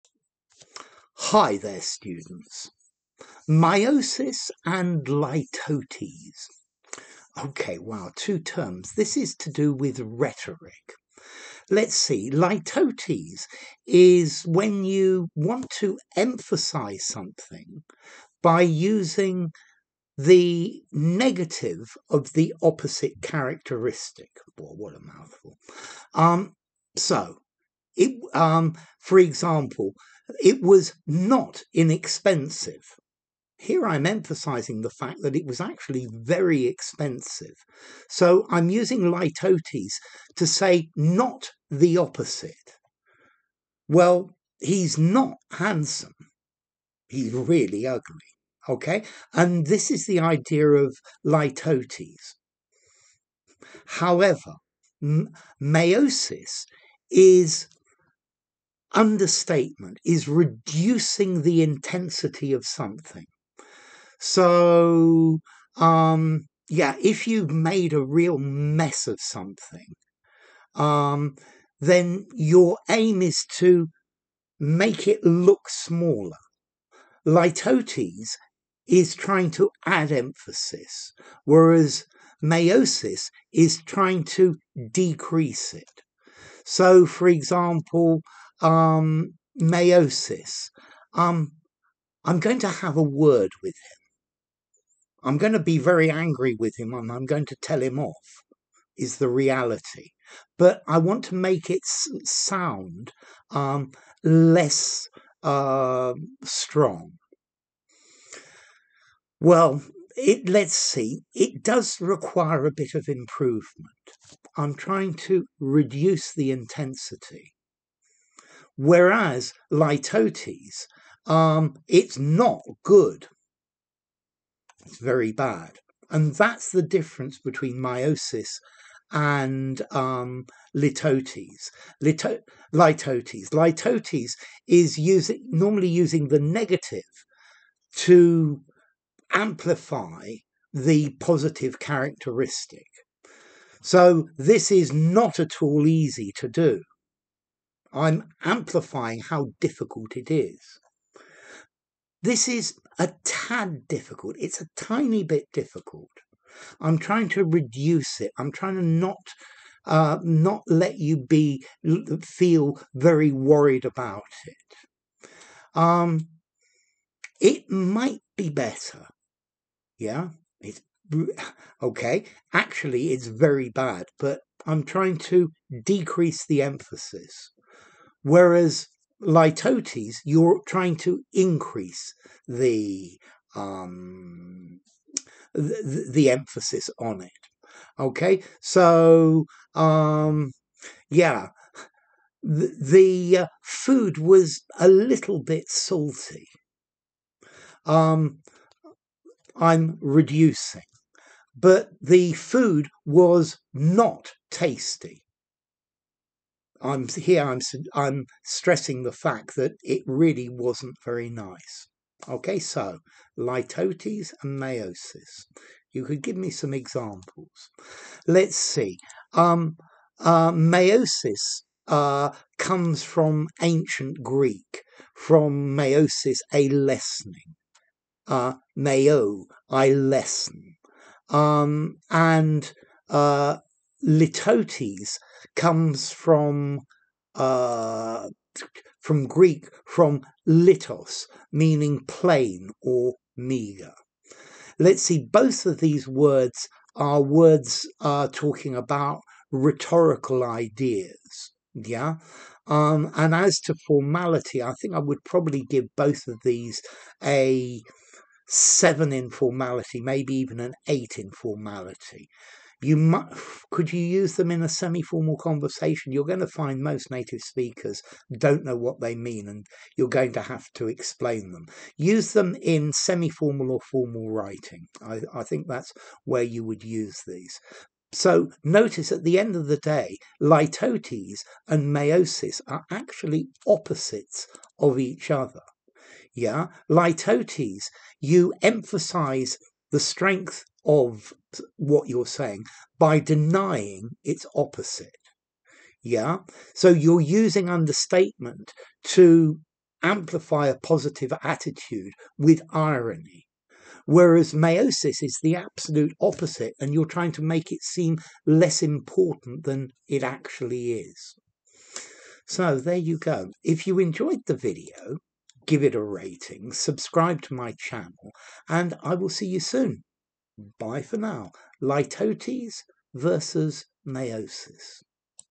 🔵 Litotes vs Meiosis - Meiosis Meaning - Litotes Definition - Rhetoric - Meiosis vs Litotes
What is meiosis. What is litotes? What is the difference between meiosis and litotes? The video gives a definition of meiosis and litotes and clearly shows the difference between these two rhetorical terms. The video gives examples of Litotes and meiosis for you to be able to understand the difference and for you to be able to use both of these concepts.